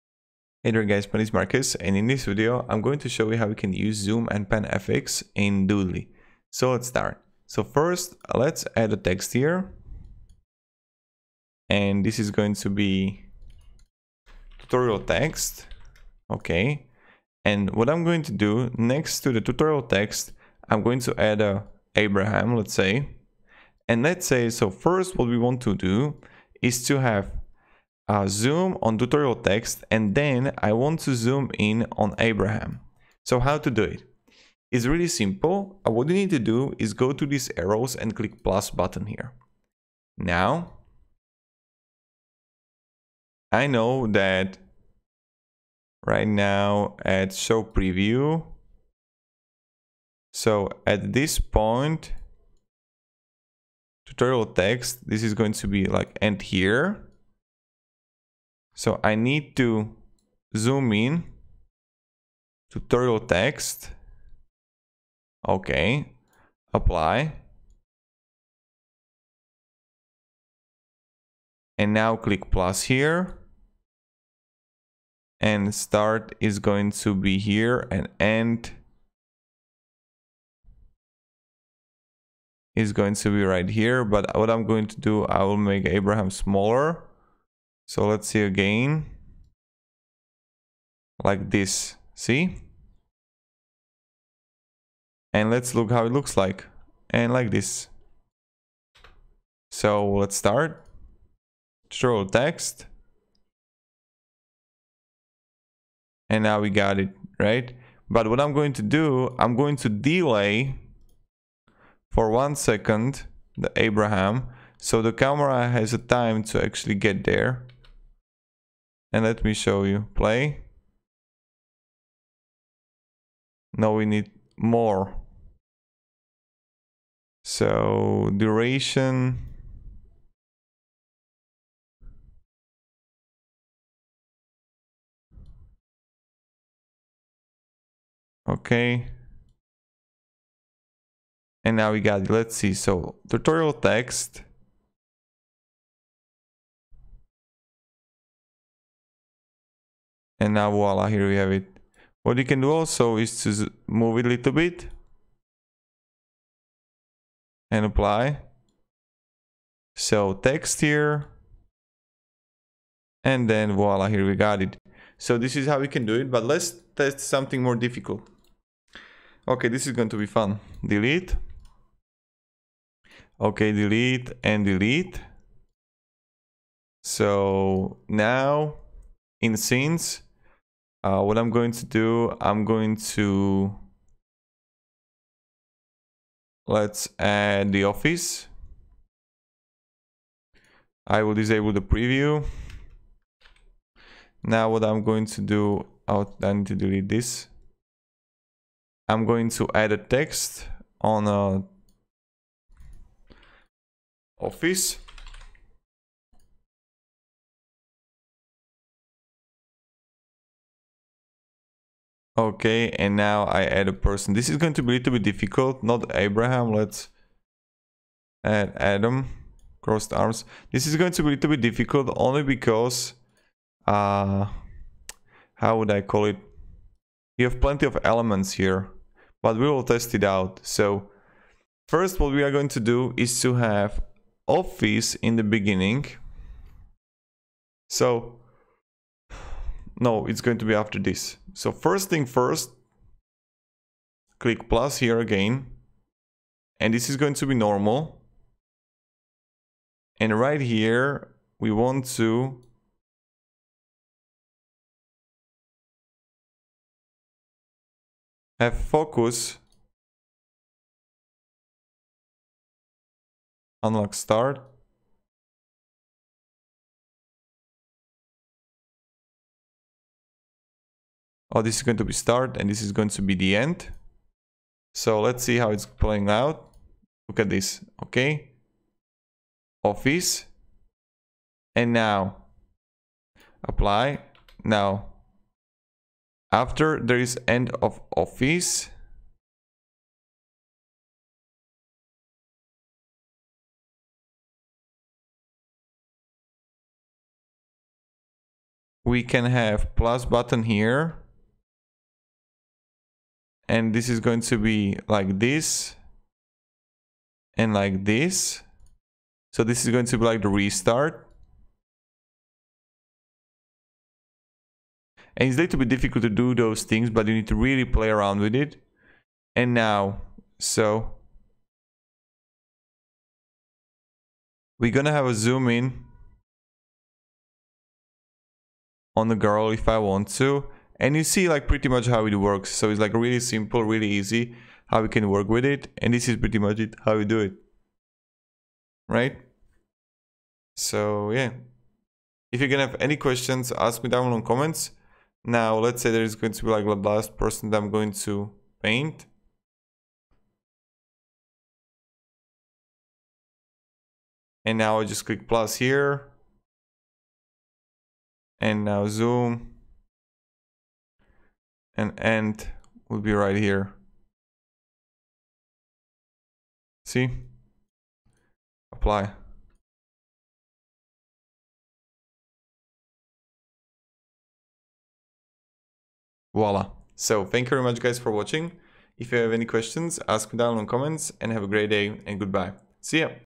How To ZOOM and PAN FX In Doodly!
In this video you will find out comparison between How To ZOOM and PAN FX In Doodly!

GET AMAZING FREE Tools For Your Youtube Channel To Get More Views:
Tubebuddy (For GROWTH on Youtube):